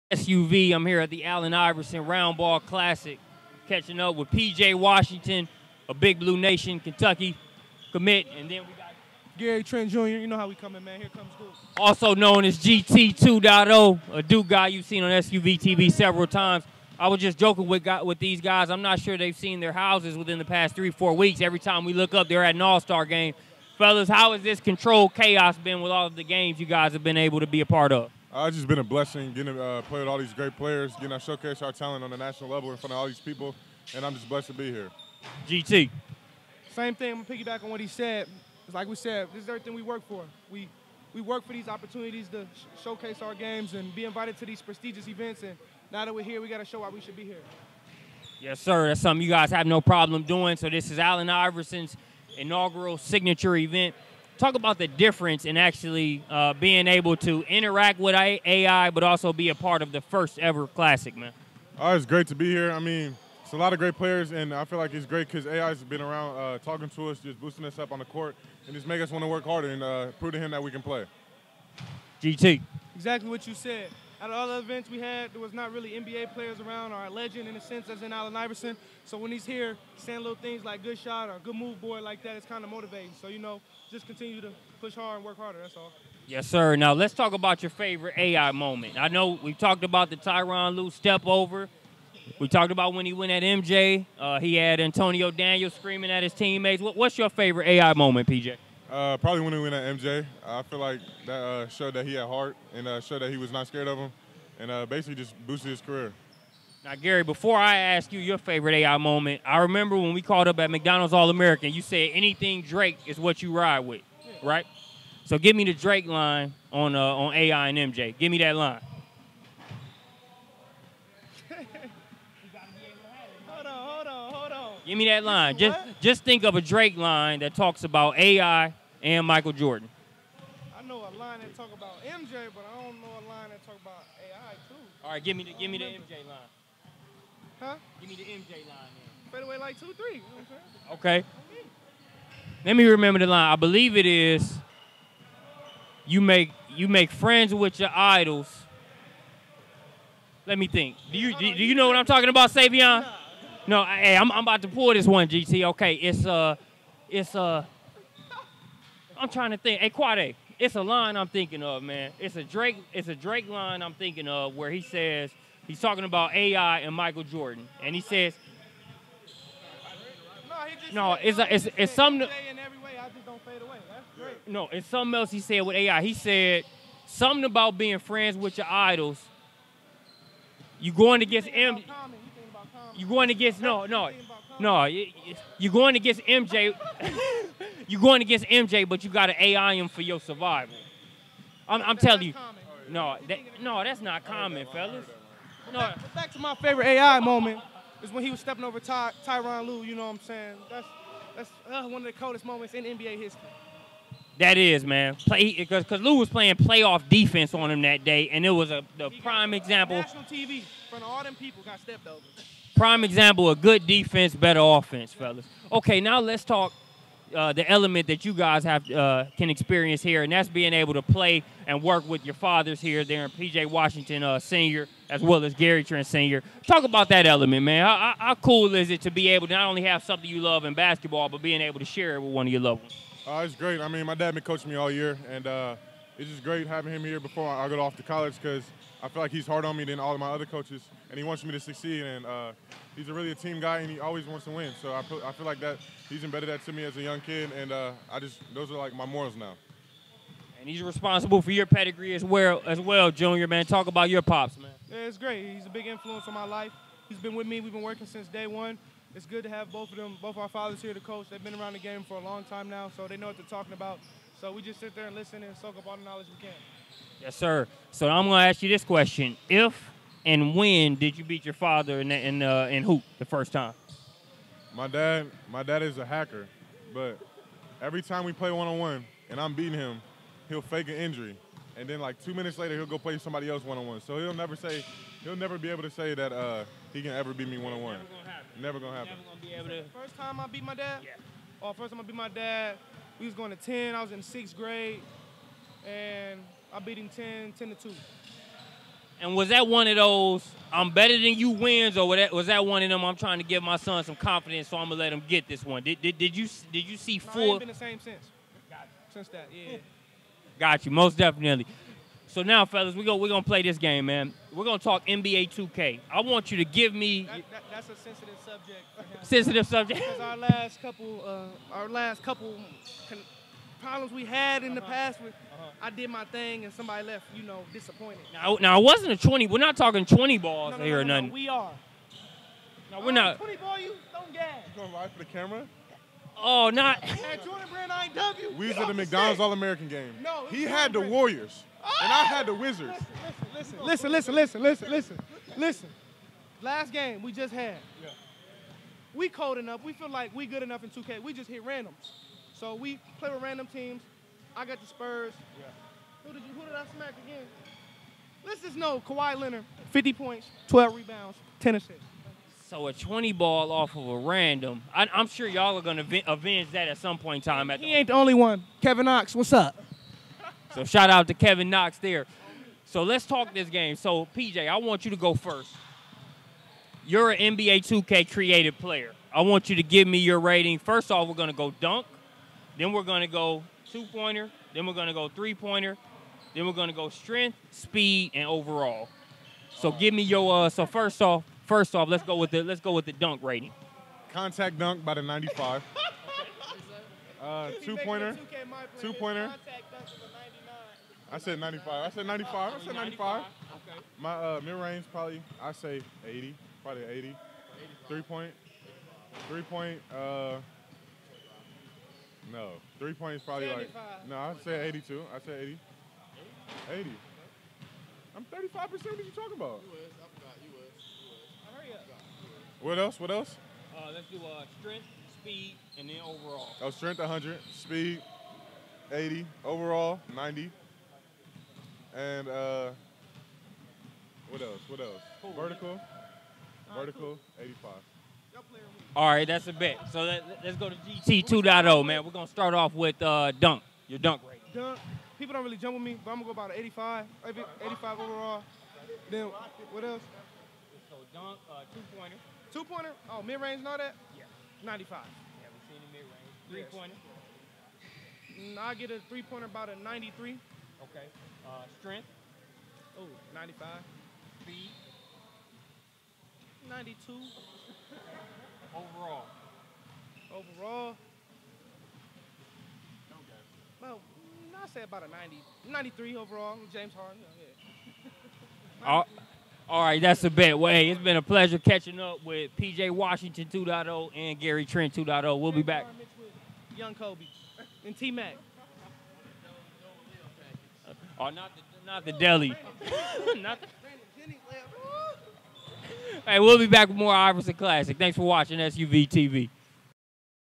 Gary Trent Jr. & P.J. Washington Interview: Allen Iverson Roundball Classic
SUVtv sits down with Gary Trent Jr. & P.J. Washington in this interview at the Allen Iverson Roundball Classic